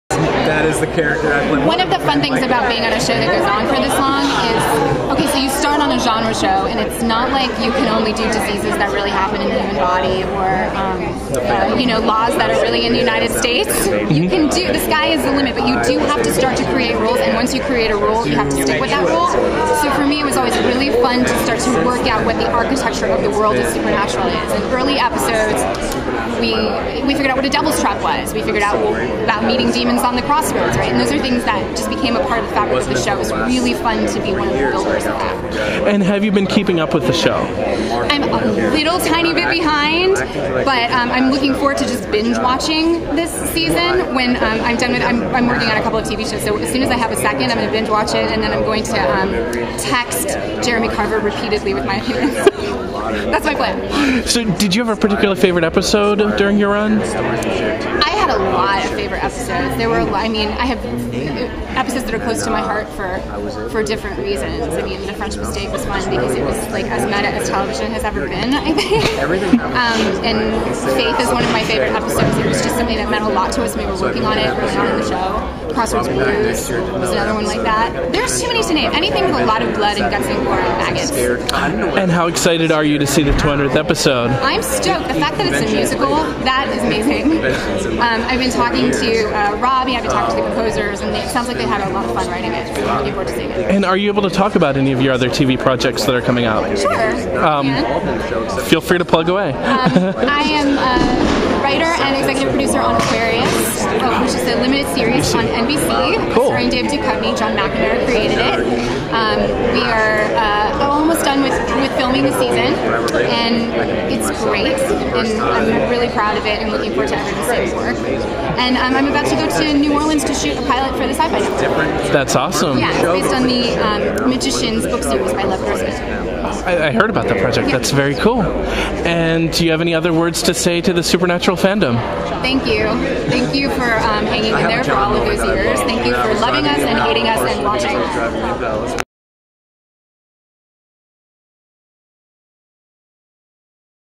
Supernatural 200th Episode Fan Party: Sera Gamble Interview
KSiteTV catches up with former Supernatural showrunner Sera Gamble at the 200th episode fan party.